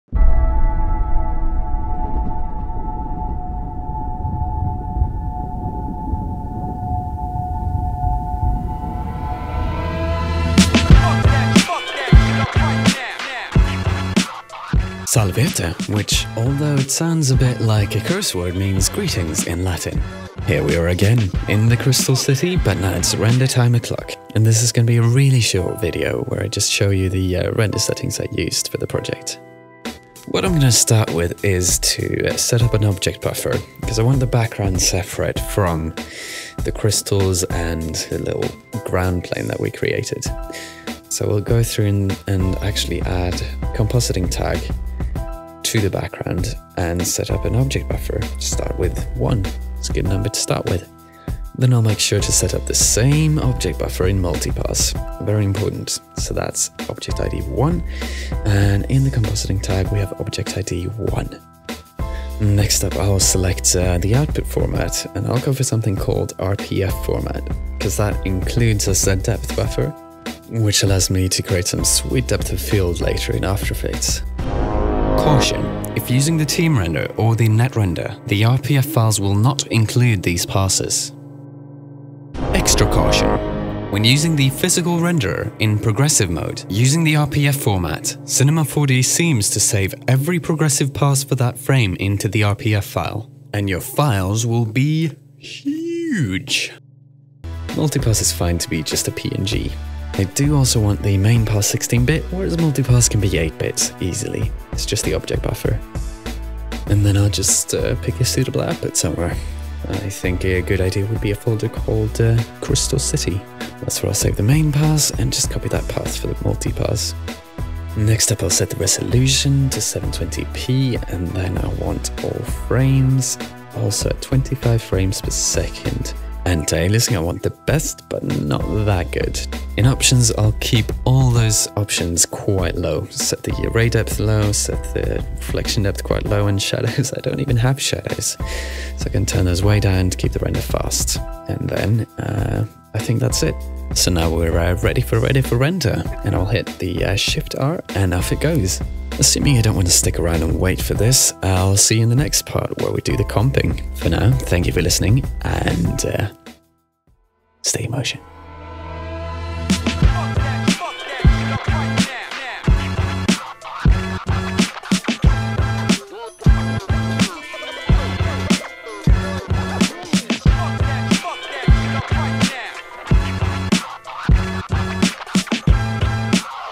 C4D TUTORIAL | Speedy Render and RPF for DOF | Crystal City PT4 [Cinema 4D]
Part 4 of 5 (getting close now!)
Now it's finally render-time. So we'll set up an object buffer, tweak our render settings for a quicker render. I also say a few wise words about using RPF-files for Depth of Field. Specifically with Team Render. (Don't.)

Sound FX
Orchestra hit: